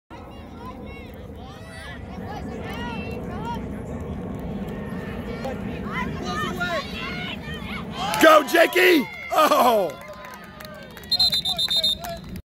Jake with a long reception for a first down.
Daylin to Jake for a first down reception.  Titans vs Saints on 9-18-21.  Titans win 33-12.